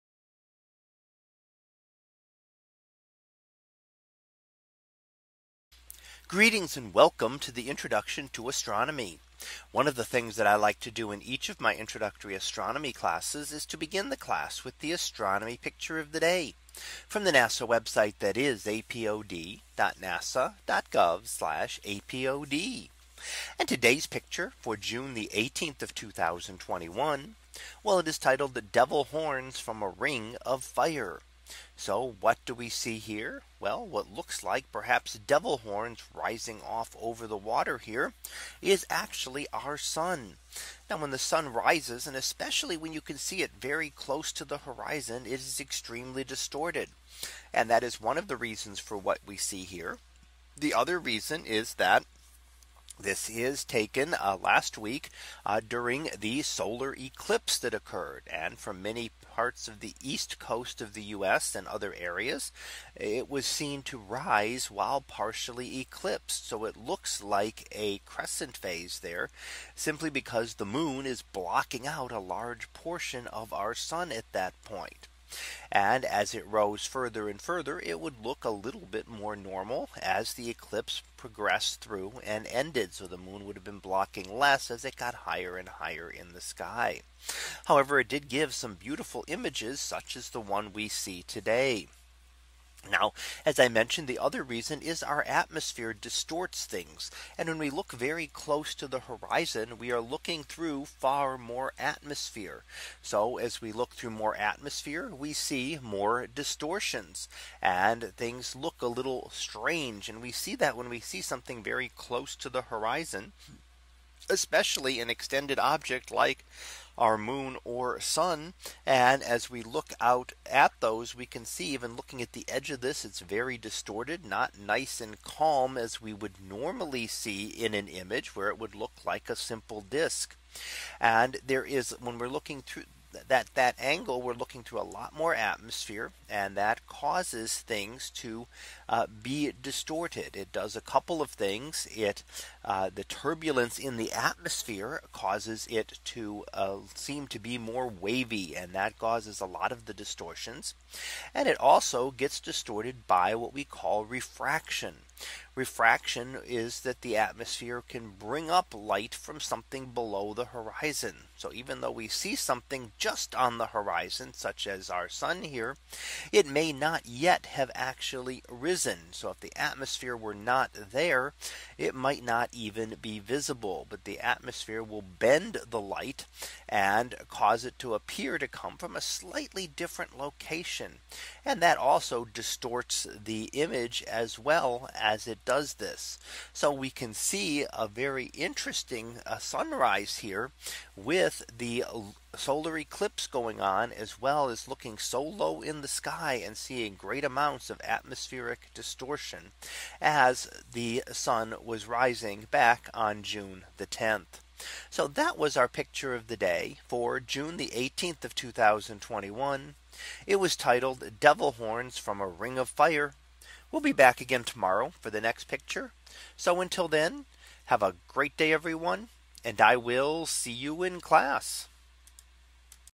2021 June 18 - Devil Horns from the Ring of Fire
In today's image, we see our Sun rising partially eclipsed from last week. We see it very distorted as well since we are looking at it close to the horizon and through a large amount of atmosphere.